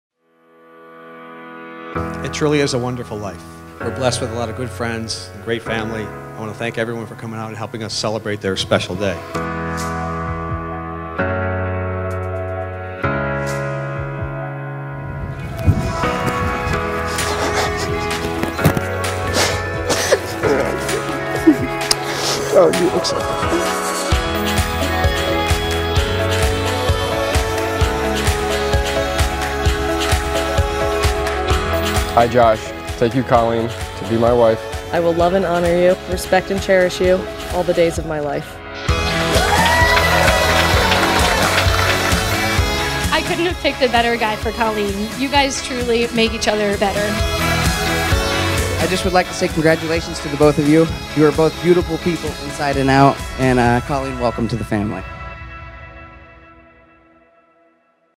Josh & Colleen's Sneak Peek
Wedding Ceremony & Reception at Silver Creek Country Club in Hellertown, PA.